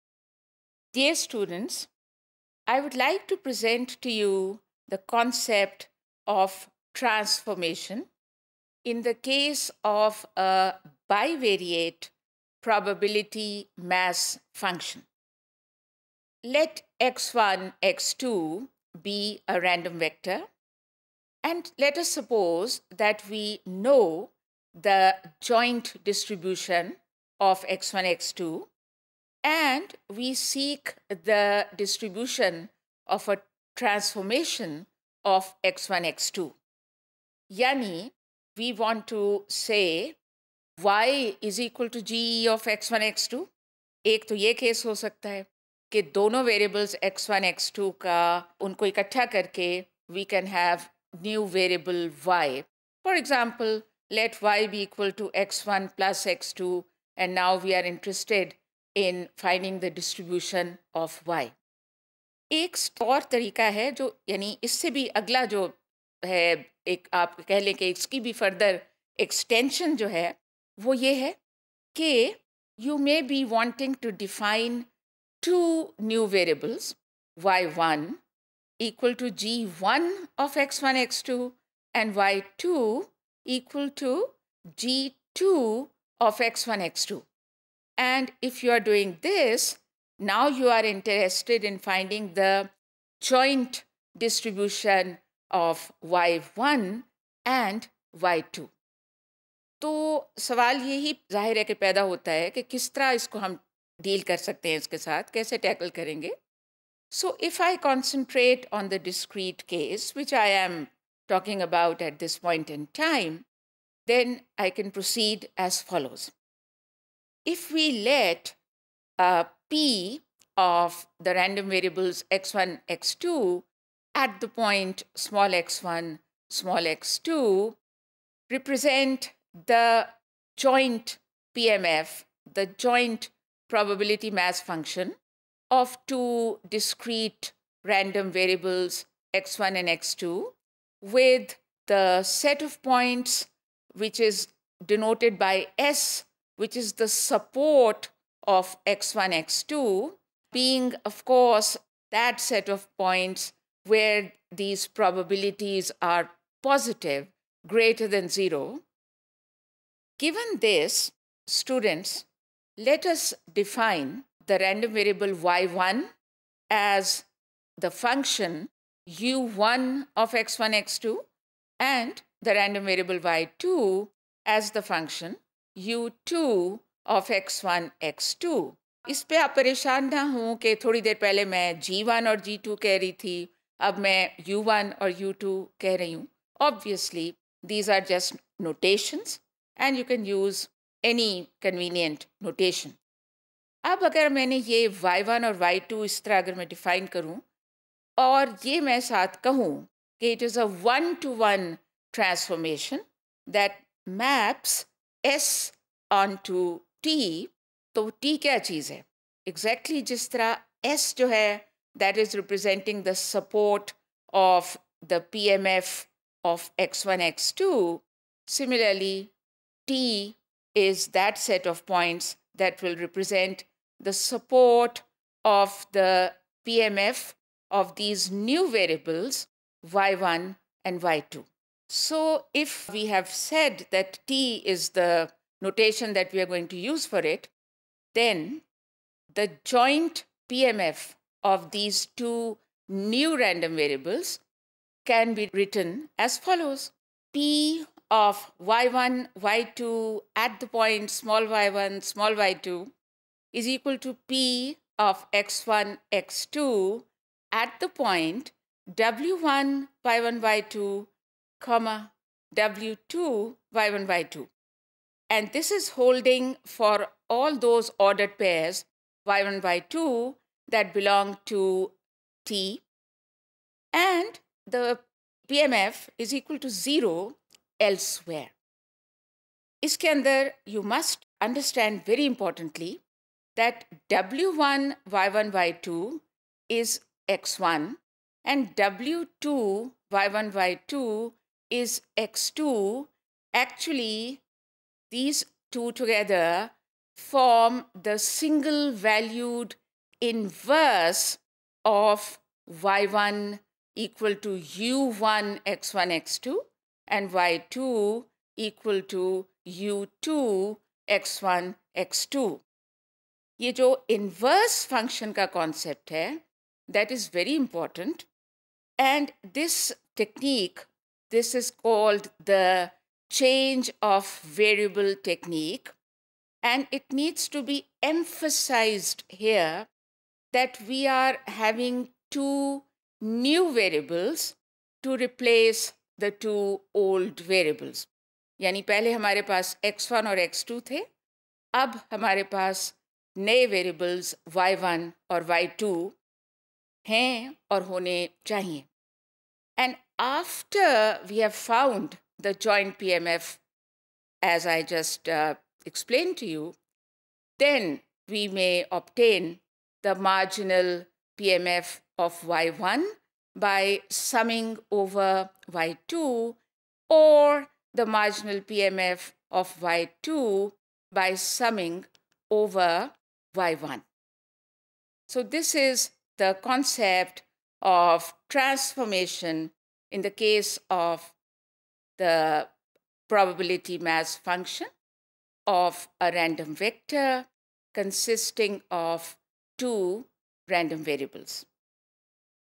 Transformation of Bivariate Probability Mass Function | Probability Distributions | STA642_Topic094
STA642 - Probability Distributions
Topic094 - Transformation of a Bivariate Probability Mass Function - Example 1
by Dr.  Saleha Naghmi